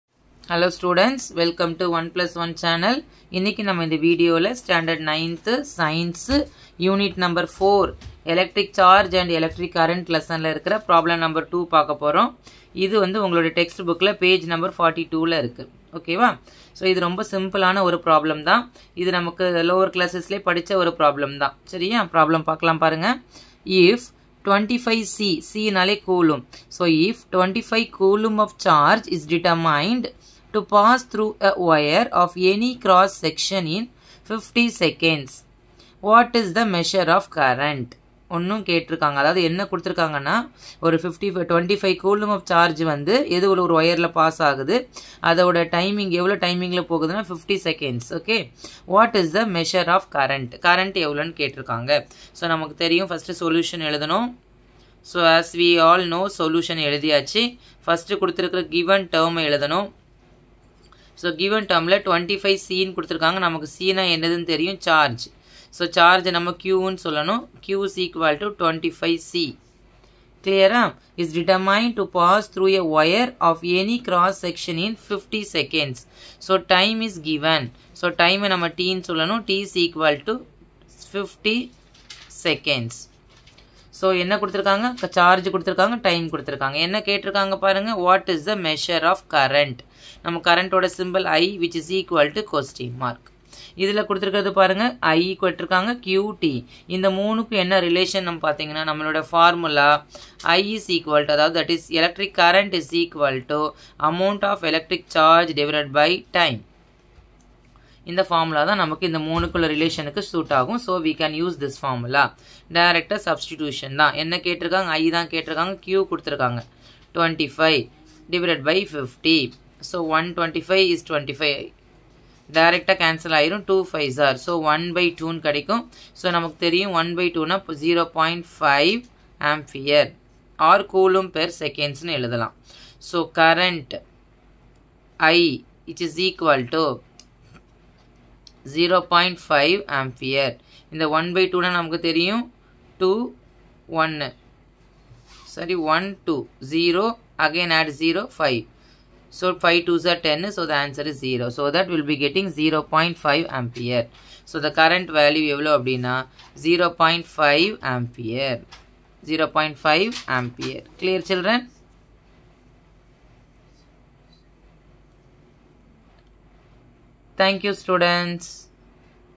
9th Science Unit-4 Electric charge&Electric current|Problem-2 Pg.No42|Samacheer One plus One channel
This video contains for 9th standard science  Unit - 4 Electric charge and Electric current, Problem number 2 Page number 42 complete solution.

My Another Channel 👇
Sangeetha Media